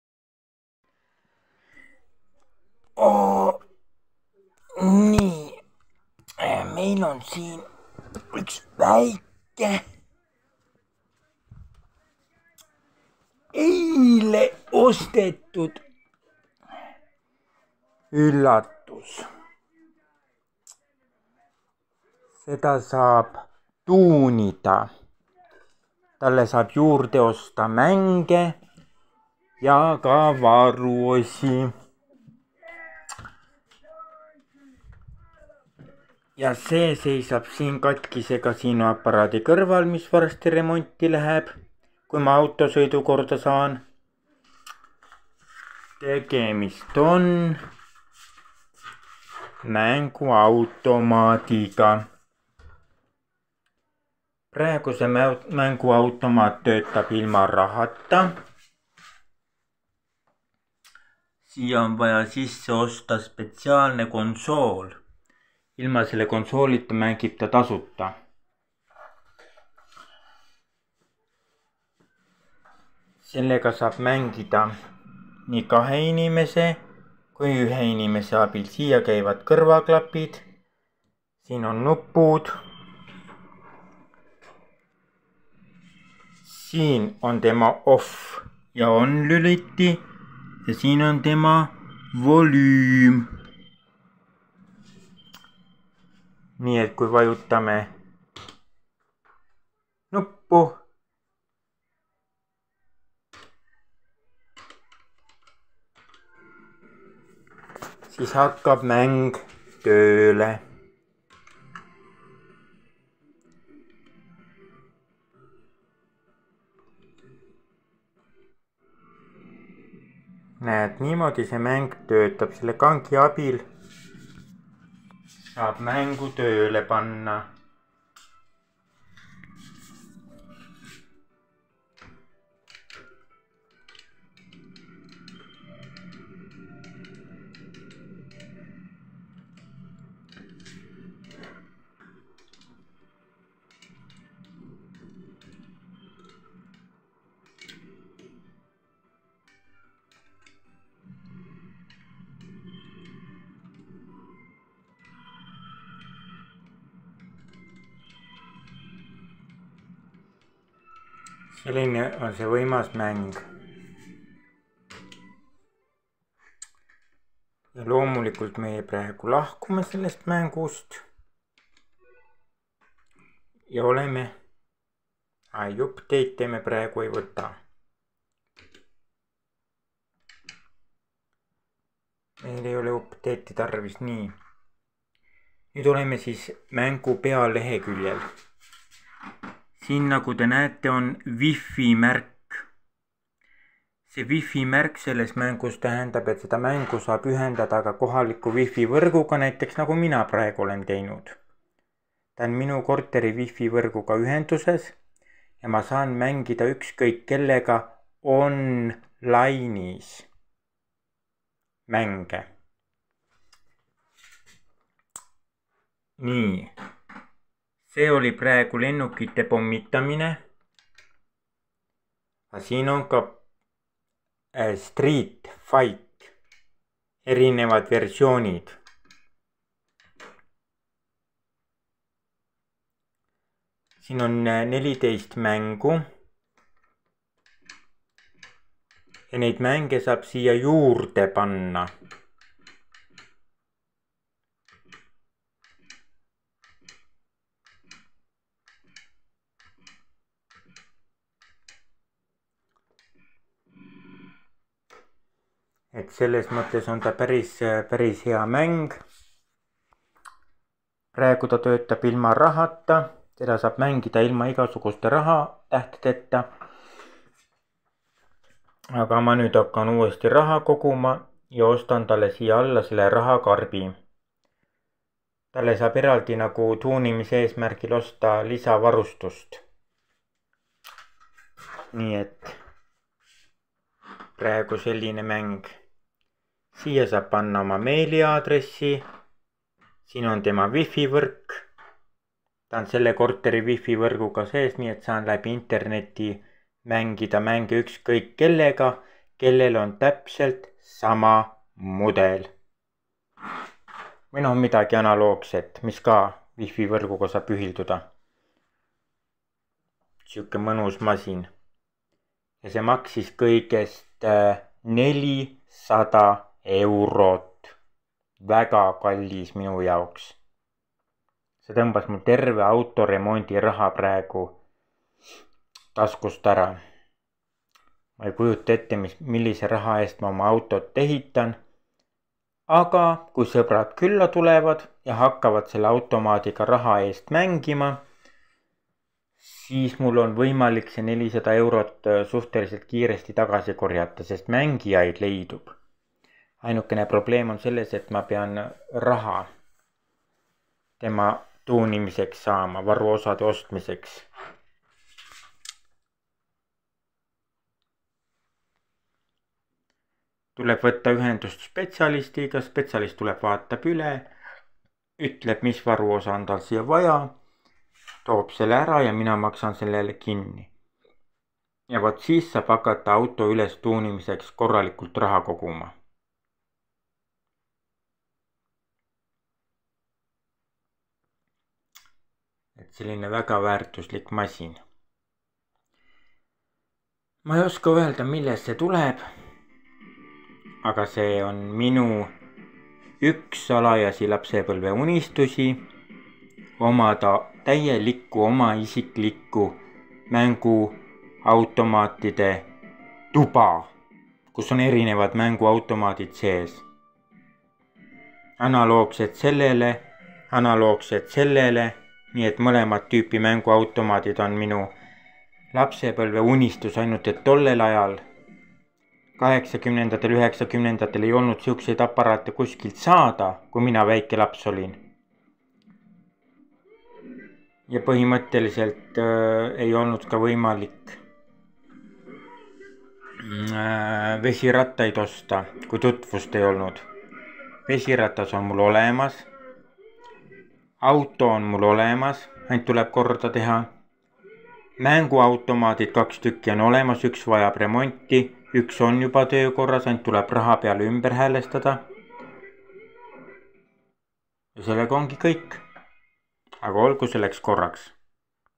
09.07.2024 Tegin ma raha kogumise tarvis olulise tųnimis väärse sişeostu
Ma ostsin eile endale lisaraha tęnimise ęsmärgil mänguautomądi miļele on peale pandud 14 erinevat mängu ja sąb neid jųrde lisada masin on upgradetav